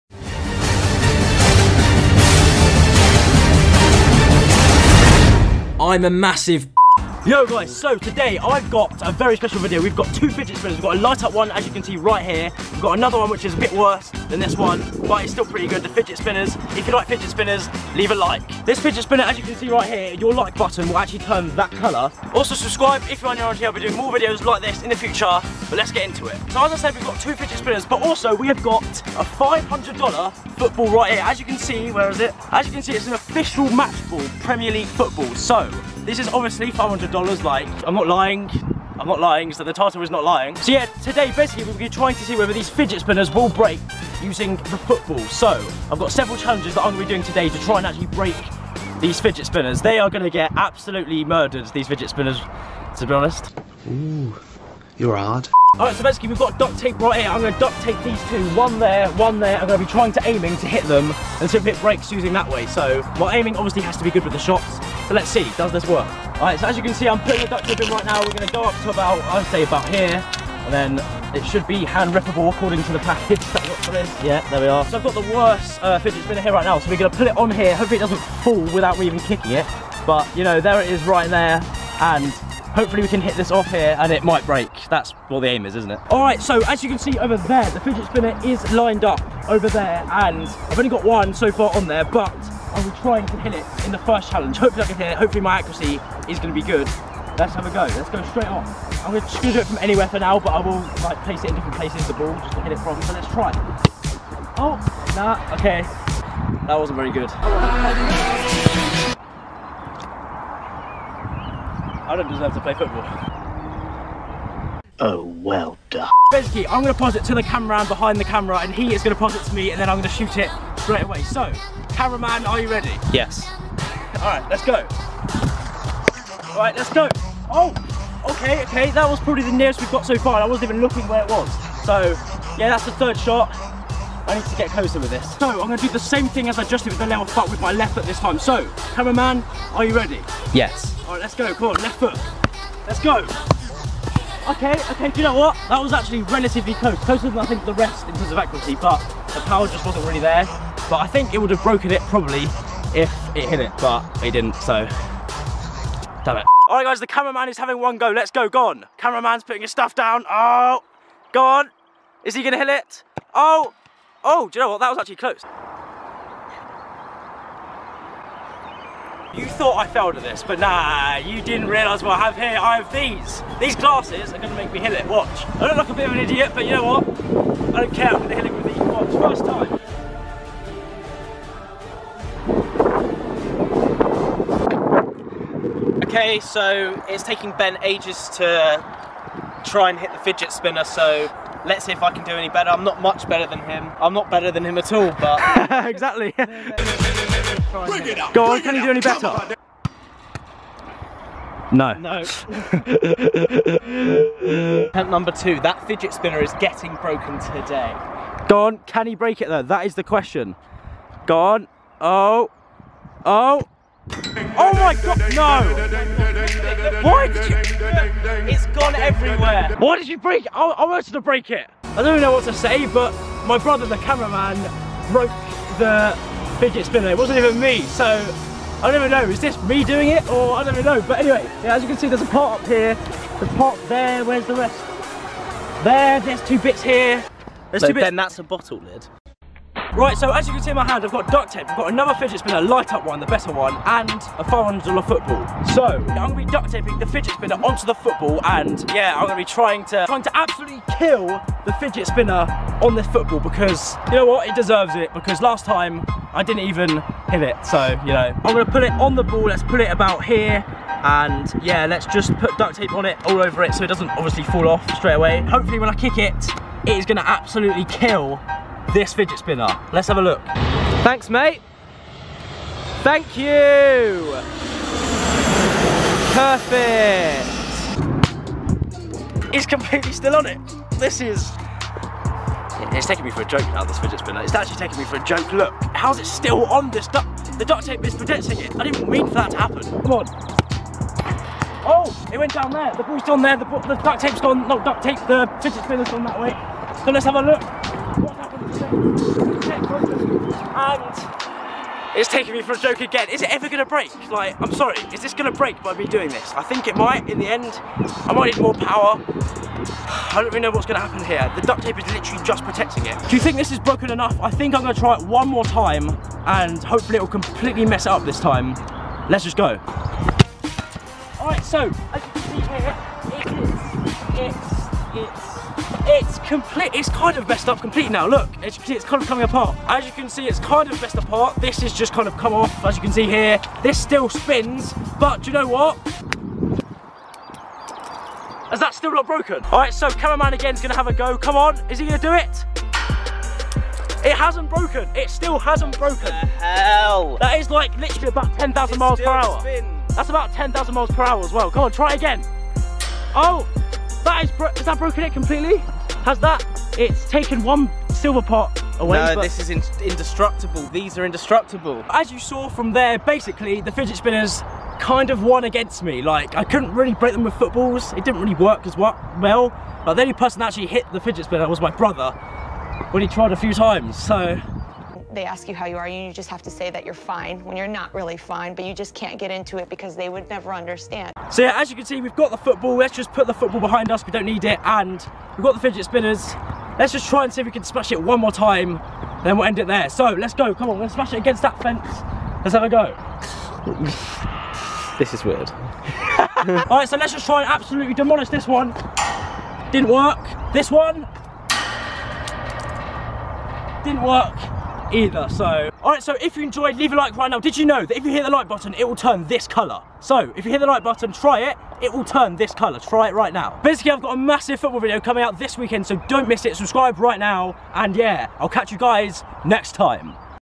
9,999+ MPH PRO FIDGET SPINNER vs. $500 FOOTBALL - is it INDESTRUCTIBLE ??
Testing out the 9,999 MPH Pro Fidget Spinner by hitting a $500 football against it in different football challenges!!! Let's see how well the Fidget Spinner keeps together

Title: 9,999+ MPH PRO FIDGET SPINNER vs. $500 FOOTBALL